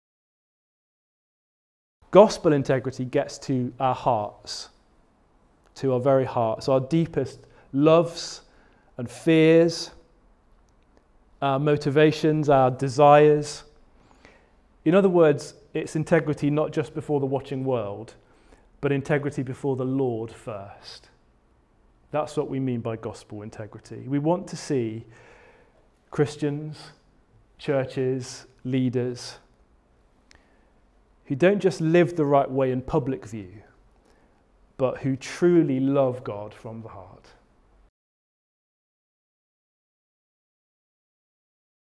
Gospel integrity comes from the Heart - Daniel Hames
Daniel Hames on gospel integrity from "Christ-Shaped Culture" session at Gathering 2023.